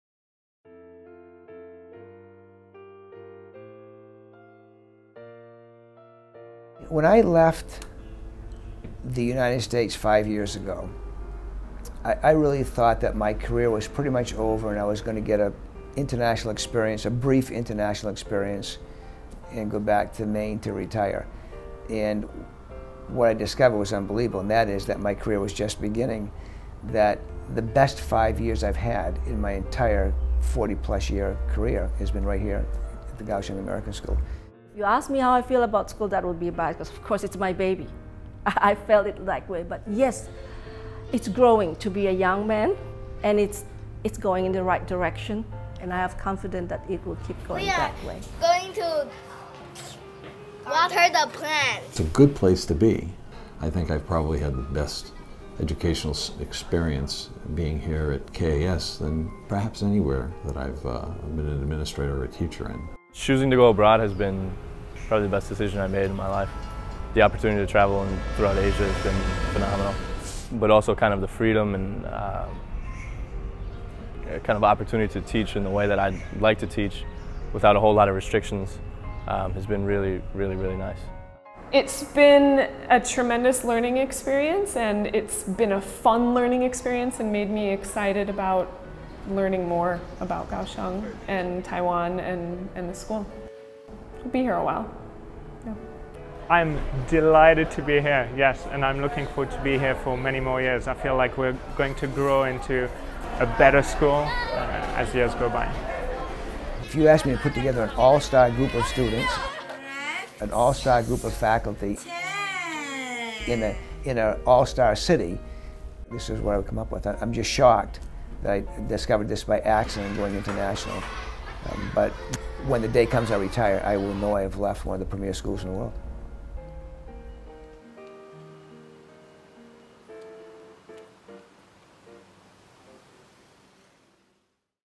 We are KAS - Part 6: Upon Reflection
We are KAS is the promotional video made for KAS by Tony Pappa of

Kaohsiung American School (KAS) is a private, non-profit Pre-K - 12 institution with just over 500 students offering college preparatory programs leading to a U.S. high school diploma. It is located in Kaohsiung, a city of 1.5 million in southwestern Taiwan.